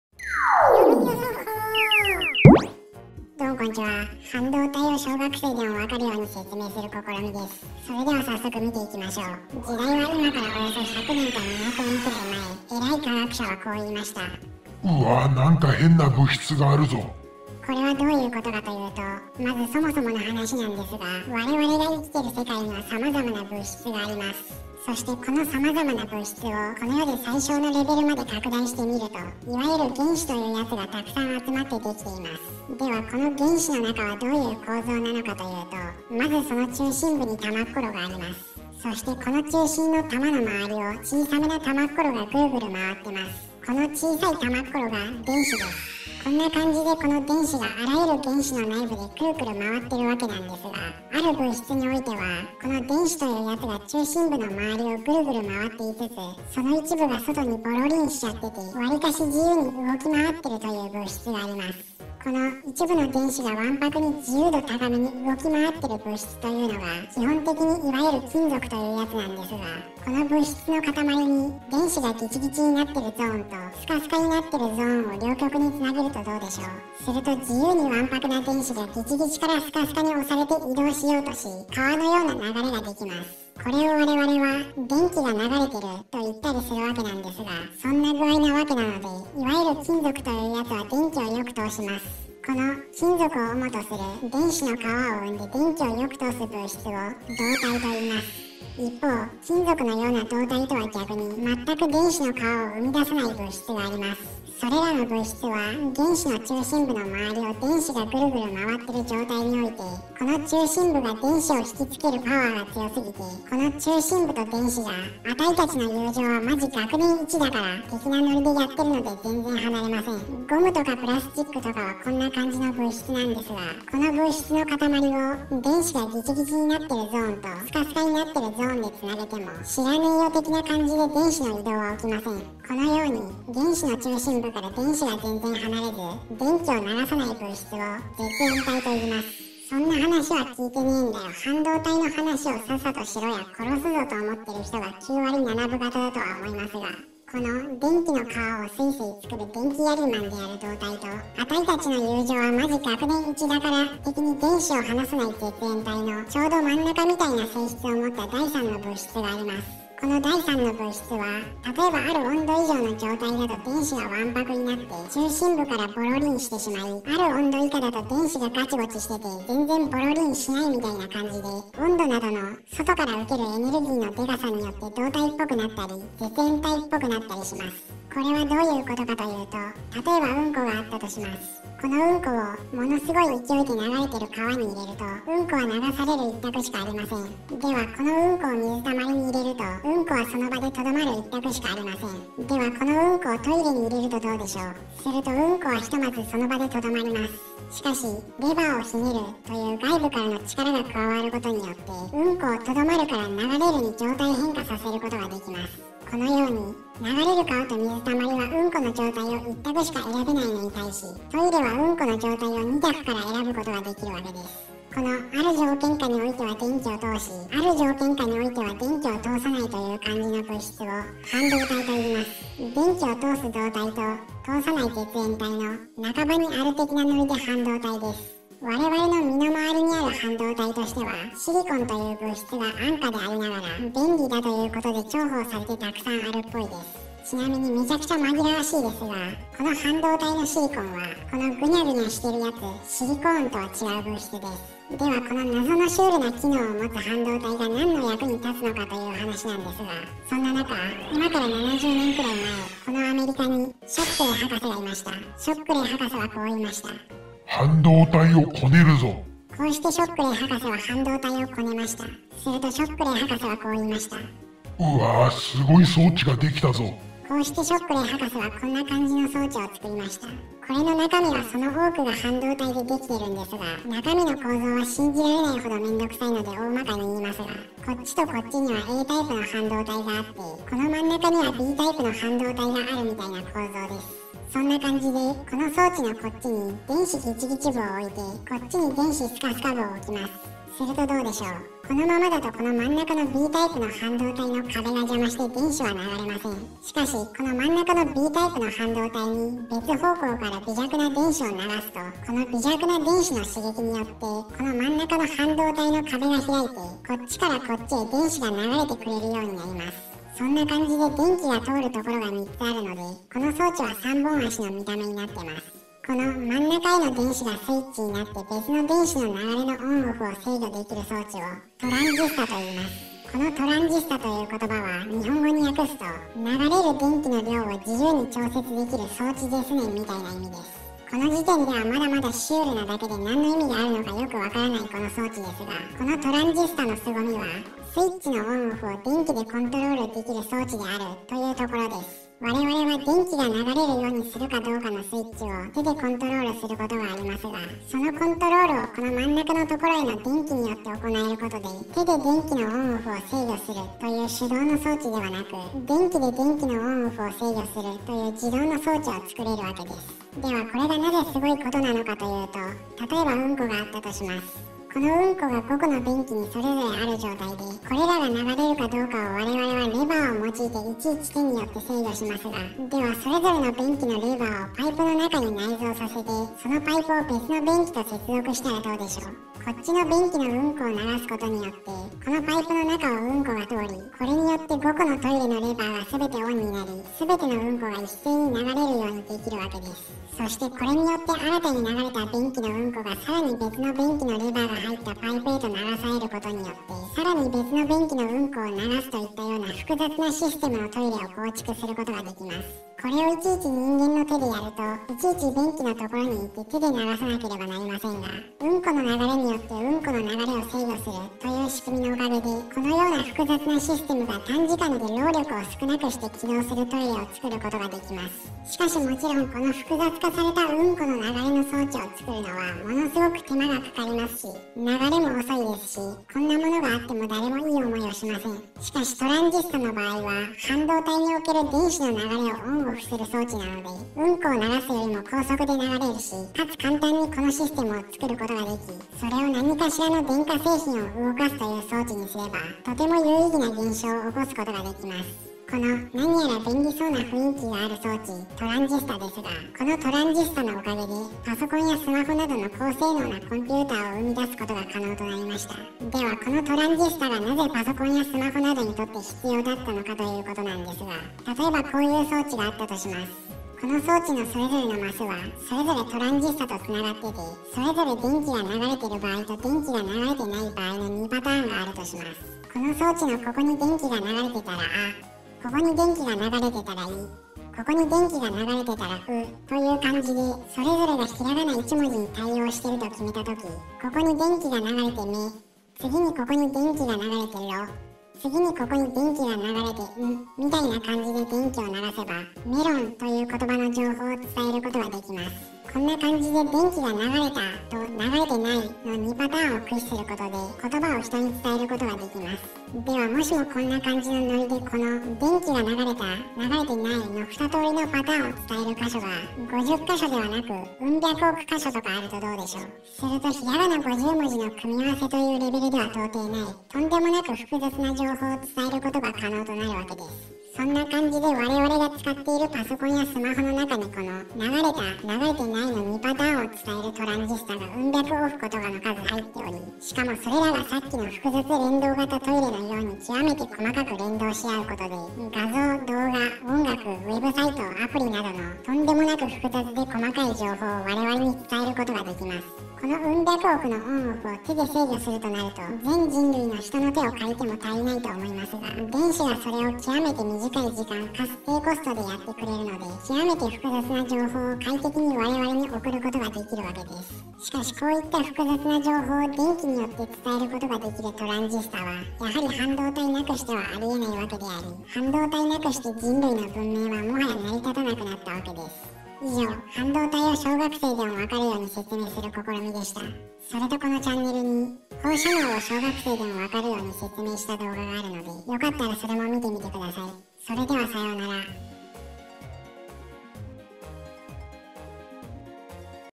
小学生でもわかる・半導体とは何か
我々がスマホを使っているのではなく、スマホが我々を使っているのだ

・その他用語
原子の中心の玉っころ・・・原子核
ボロリンしちゃっててわりかし自由な電子・・・自由電子
電子がスカスカになってるゾーン・・・プラス極
電子がギチギチになってるゾーン・・・マイナス極
今回動画で紹介したタイプのトランジスタ・・・バイポーラトランジスタ（NPN型）
Ａタイプの半導体・・・N型半導体
Ｂタイプの半導体・・・P型半導体
トランジスタの左側・・・エミッタ
トランジスタの真ん中・・・ベース
トランジスタの右側・・・コレクタ